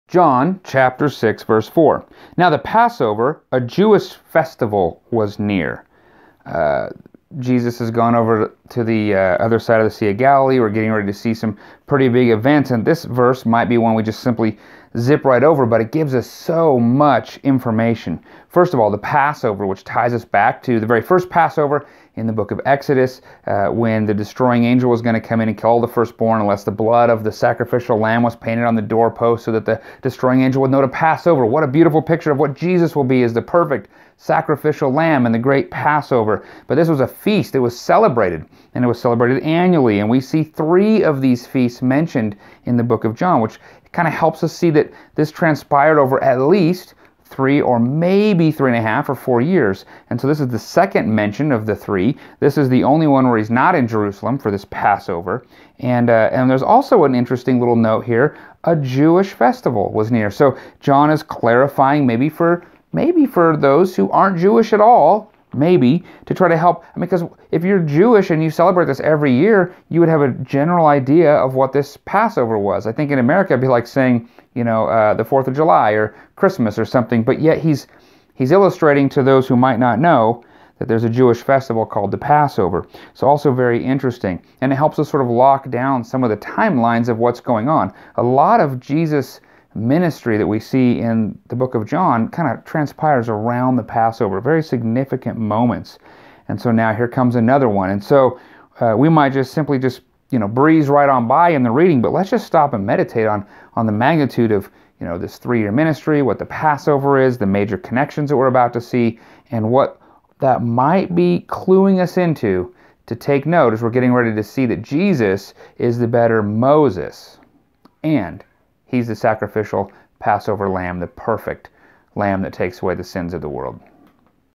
Journey Through John -- John 6:4
Bryan Catherman offers two minutes on John 6:4.  "Now the Passover, a Jewish festival was near."